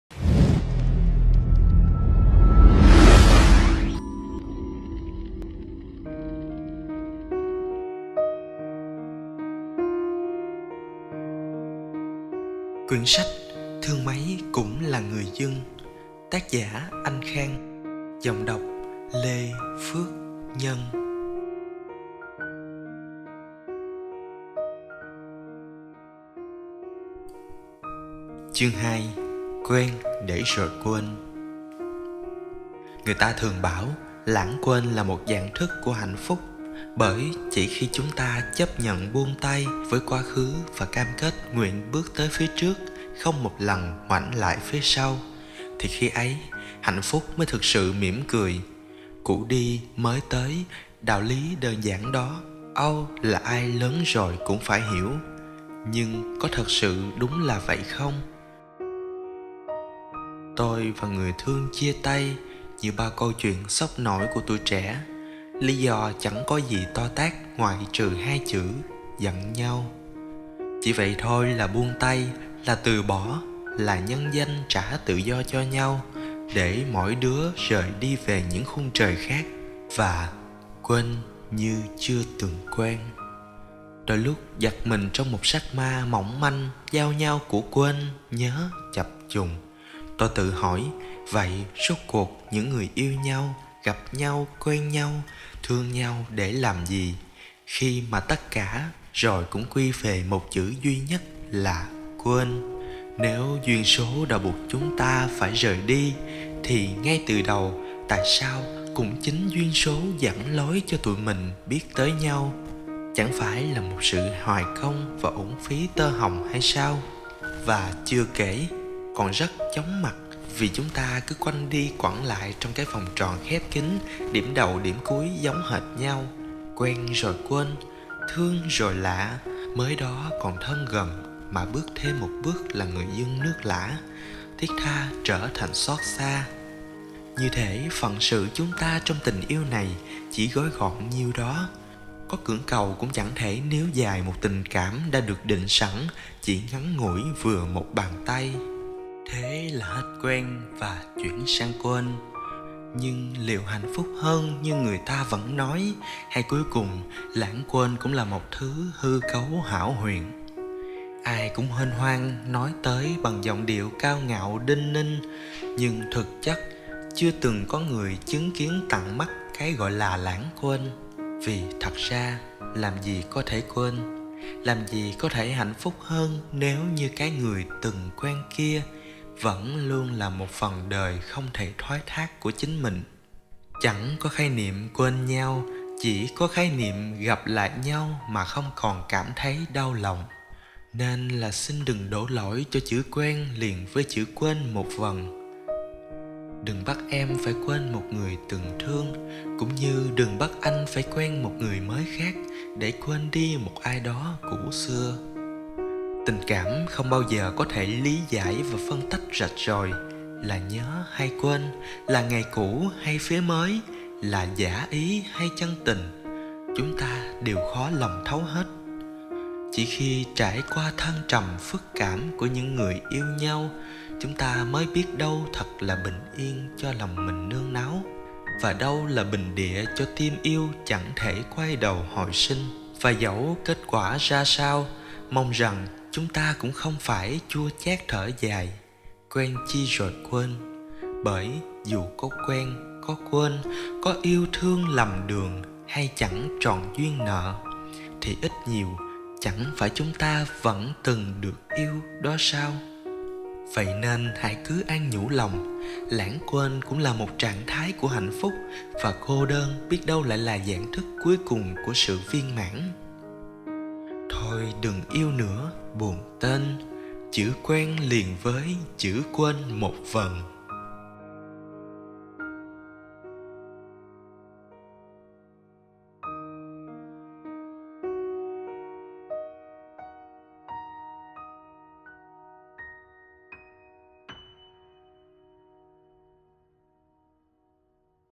Thương Mấy Cũng Là Người Dưng|Anh Khang - Chương 2: Quen Để Rồi Quên
/Chuyên mục luyện đọc/
Thương Mấy Cũng Là Người Dưng - Tác Giả: Anh Khang
Chương 2: Quen Để Rồi Quên
Giọng Đọc: Lê Phước Nhân